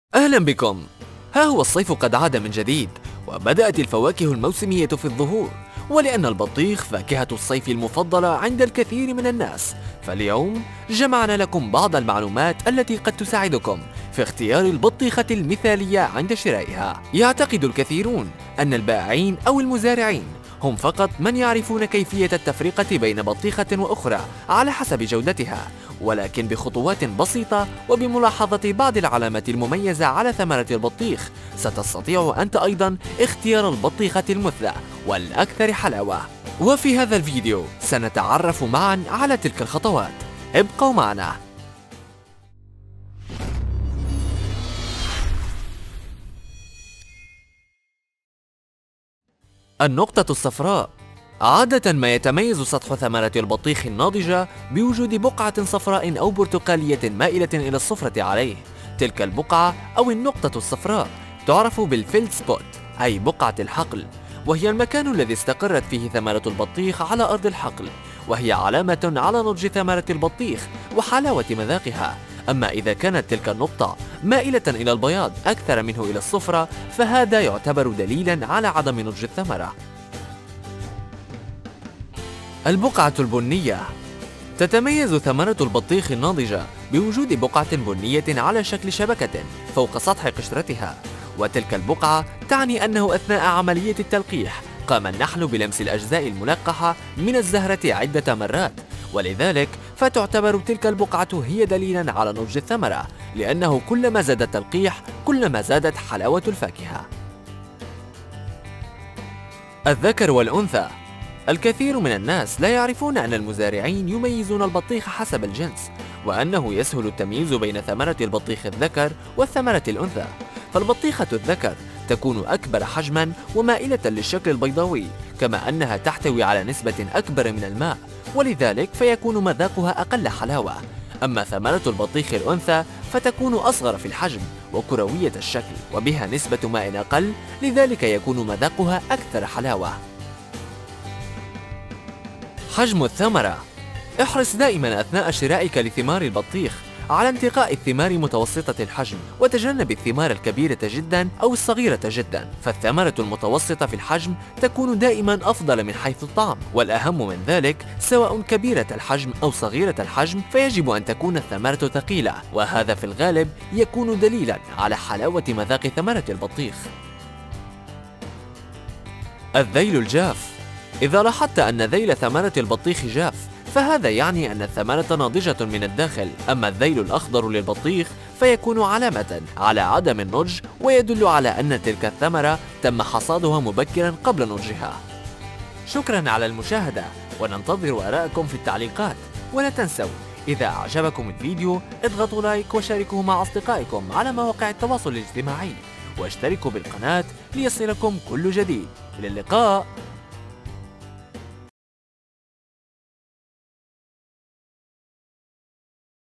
كيف تميز البطيخة الناضجة عن غيرها ؟!
كيف تعرف البطيخة الناضجة من غيرها , ها هو الصيف قد عاد من جديد، وبدأت الفواكه الموسمية فى الظهور، ولأن البطيخ فاكهة الصيف المفضلة عند كثير من الناس، فاليوم جمعنا لكم بعض المعلومات التي قد تساعدكم في اختيار البطيخة المثالية عند شرائها، يعتقد الكثيرون ان البائعين او المزارعين هم فقط من يعرفون كيفية التفرقة بين بطيخة وأخرى على حسب جودتها، ولكن بخطوات بسيطة وبملاحظة بعض العلامات المميزة على ثمرة البطيخ ستستطيع انت ايضا اختيار البطيخة المُثلى والاكثر حلاوة فى الطعم، وفى هذا الفيديو سنتعرف معا على تلك الخطوات، ابقوا معنا.

شاهد أيضا المزيد من فيديوهاتنا .